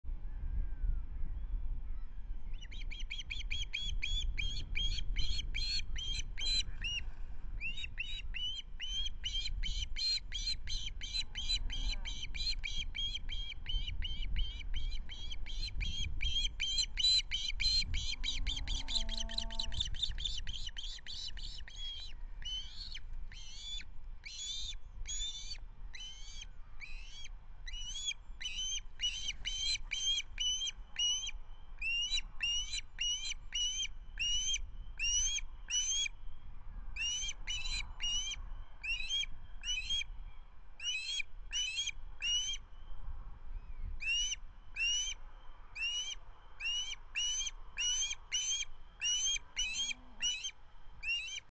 Osprey 33(11) catching breakfast this morning
Another great shot of 33(11) catching breakfast this morning (look to the top left hand side of the screen), which 1R0 was more than happy to take off him

Lovely to see (and hear) our Manton Bay Osprey, and it’s shaping up to be a gorgeous weekend